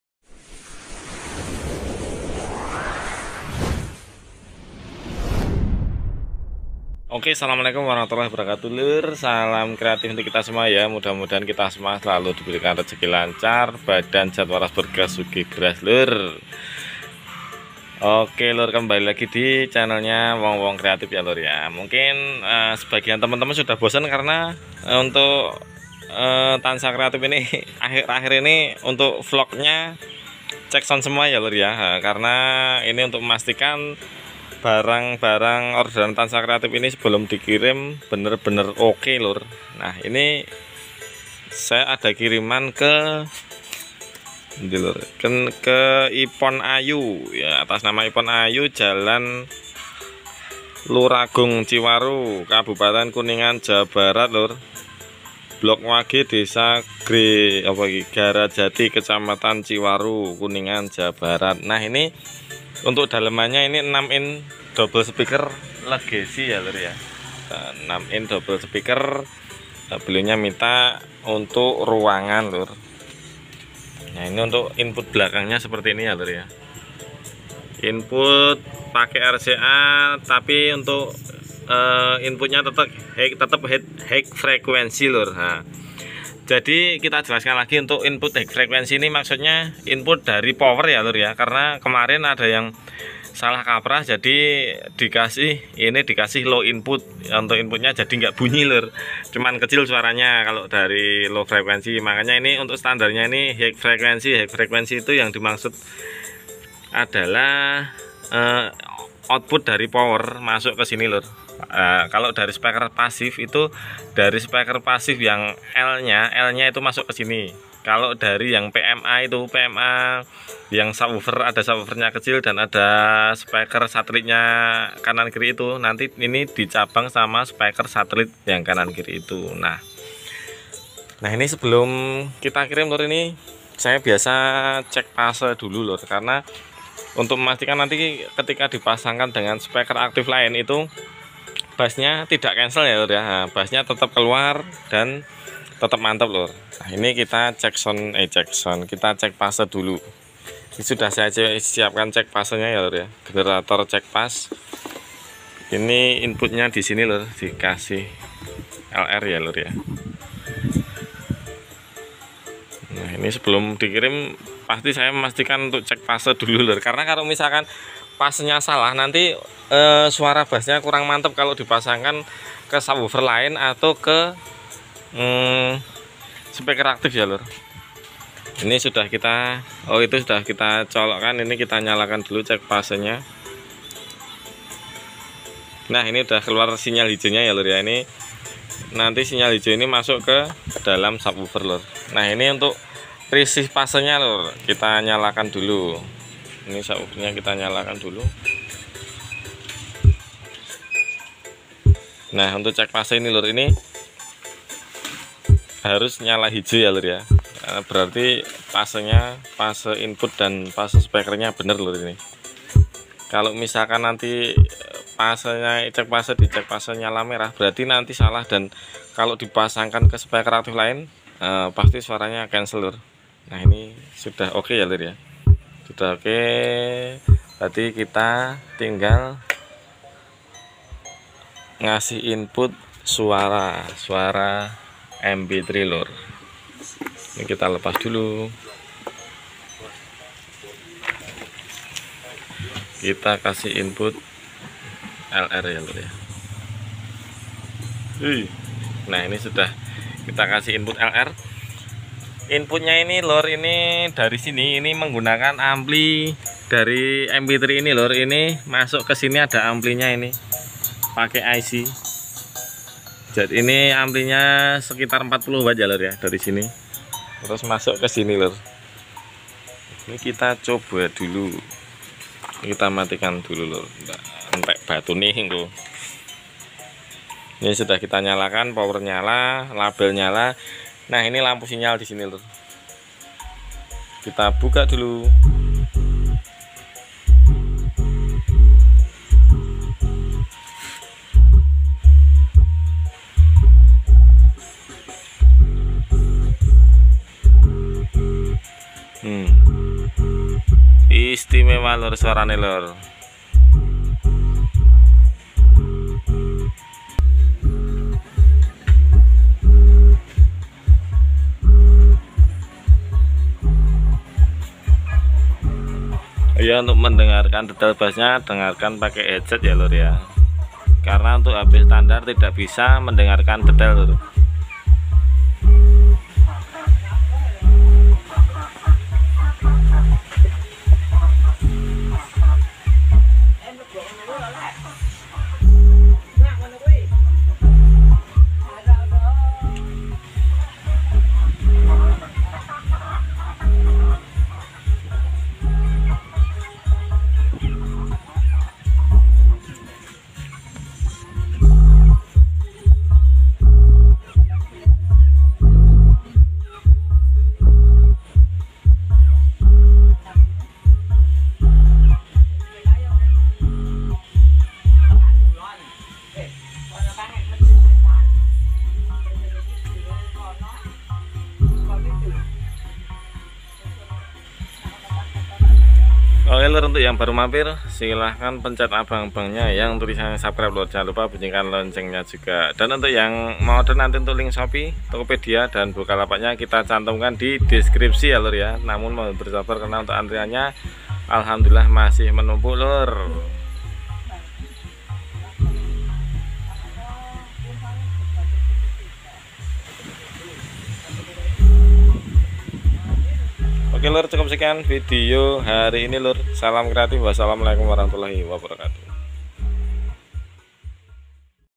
cek Sound Bazooka kirim ke: Ipon ayu - Ciwaru -Kuningan Jabar
cek sound Bazooka 6 inch dobel speaker kiriman ke kirim ke: Ipon ayu - Ciwaru -Kuningan Jabar
Untuk dulur2 yang mau order bisa klik link dibawah ini ya..